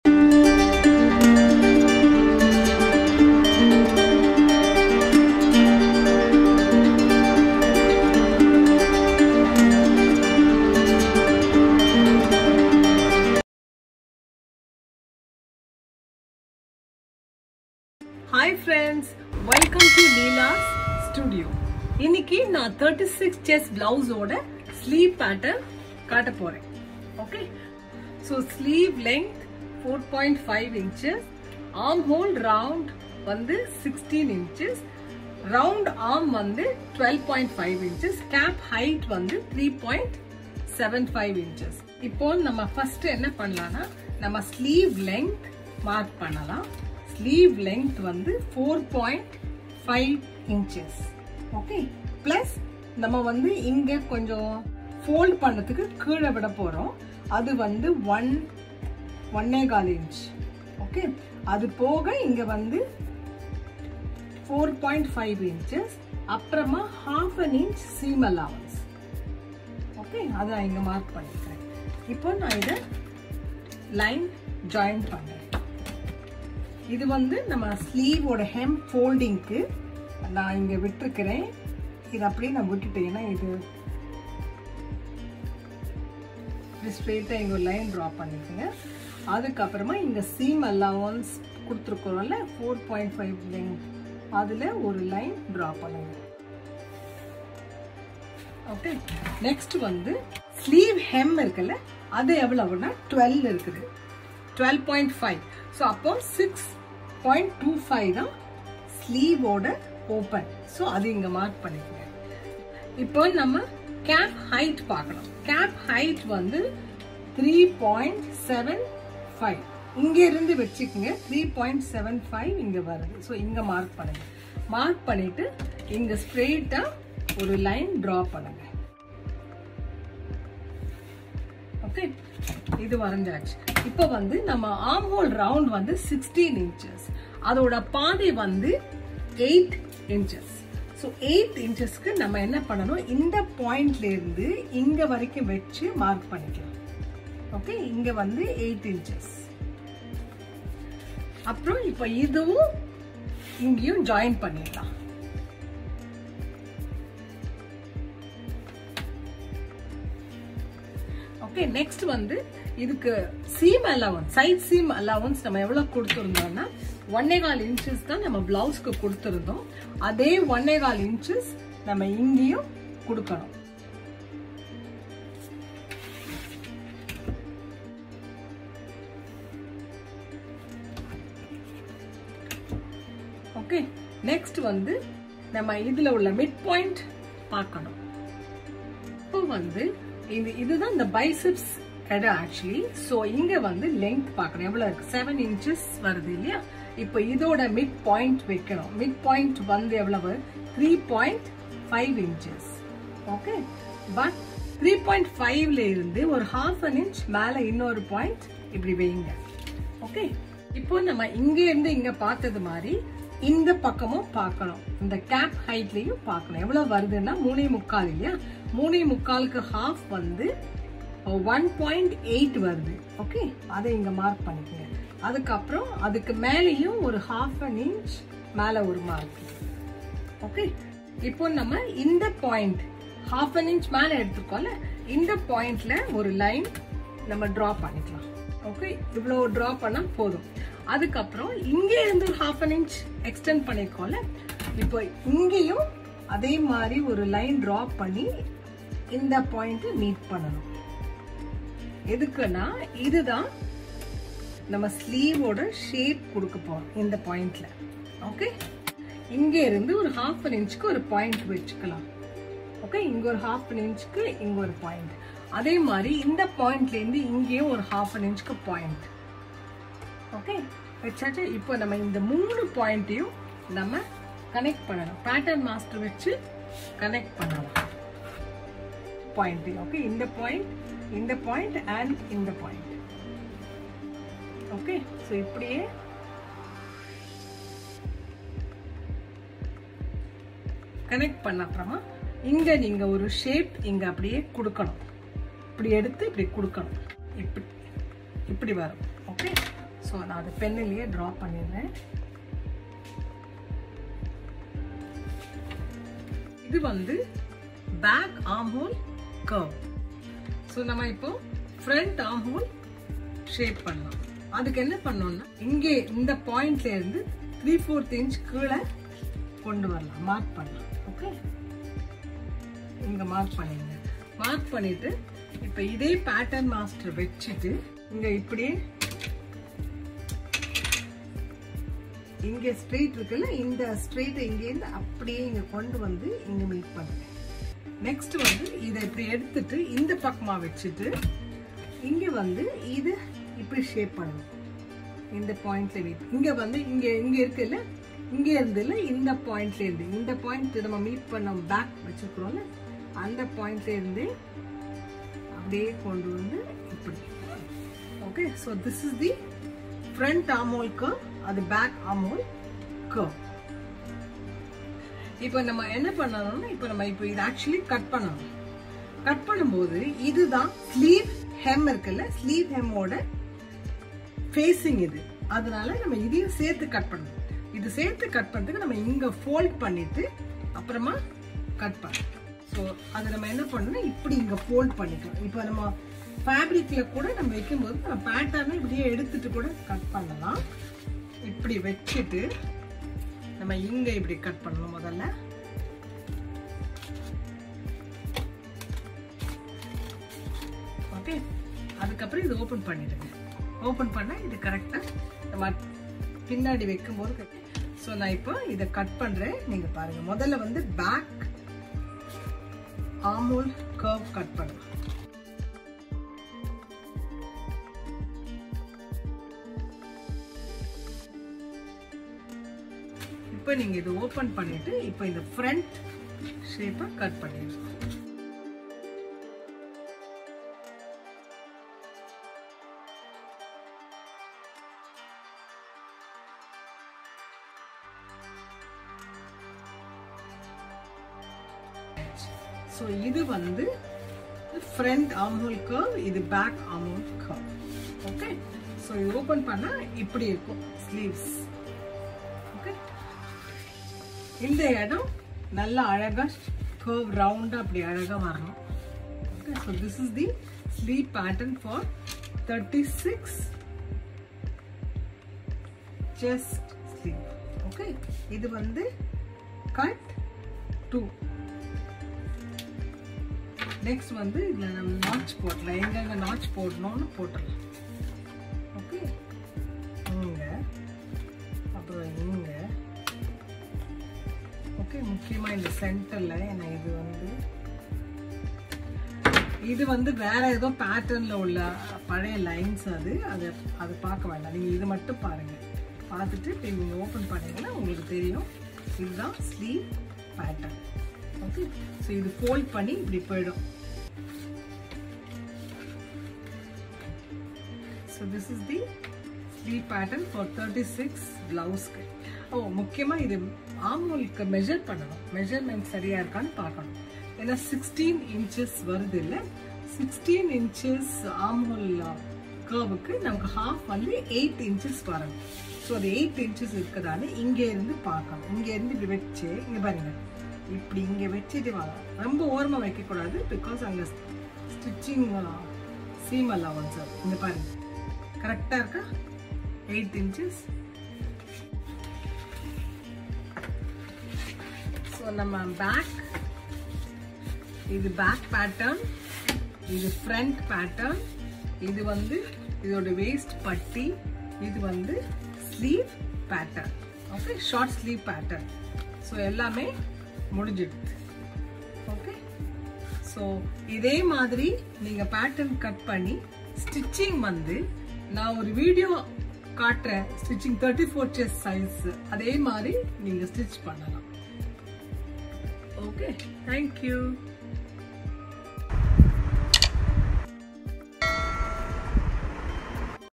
36" Size blouse pattern-Part-5 Tamil / English captions. 100% perfect cutting in a professional way.
Learn to cut Sleeve pattern for your 36" chest blouse in a professional way.

You can become a master in sewing blouse.

How to lay the pattern and cut

Video to see the blouse sewing.

How to attach sleeves and finishing of blouse.

howtocut36”blouseinaprofessionalway
#36”blousesleevepattern
sleevepatternfor36”blouse